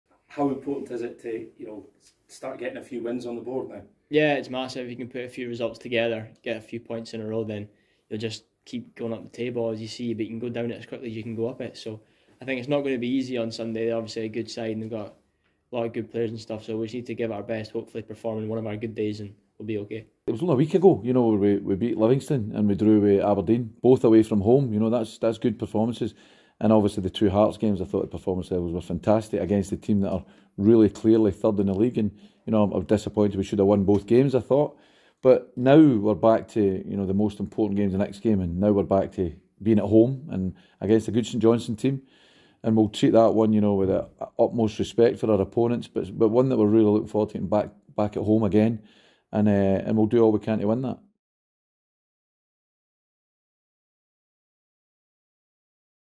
Lyall Cameron & Tony Docherty | Presser | Now Available on DeeTV
Lyall Cameron & Tony Docherty speak to the press ahead of the St Johnstone game, watch the full presser now on DeeTV